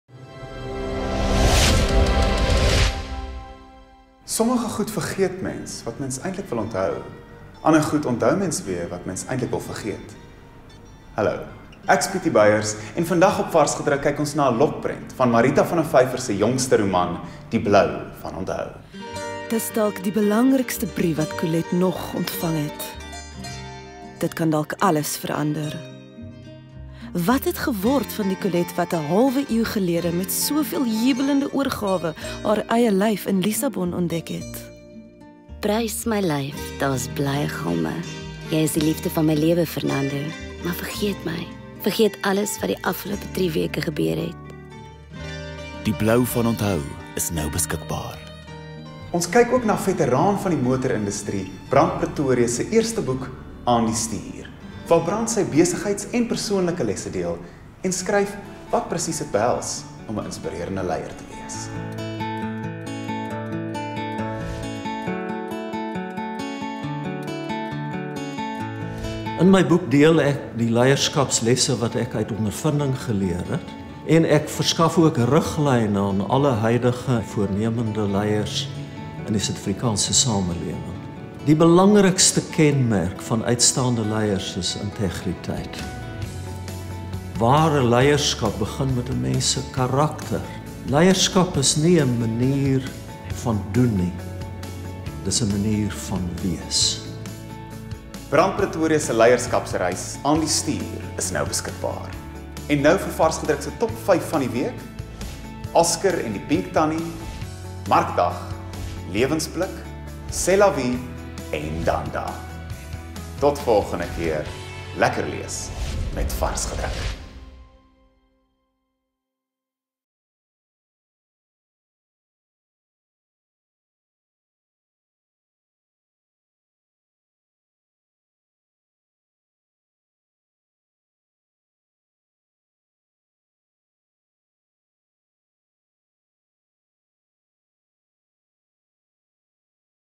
Varsgedruk 37: Brand Pretorius se Aan die stuur; en Marita van der Vyver se Die Blou van onthou.
In hierdie Varsgedruk insetsel kyk ons na die boek "Die Blou van Onthou" deur Marita van der Vyver; asook na "Aan die Stuur" deur Brand Pretorius

Lekker lees met Varsgedruk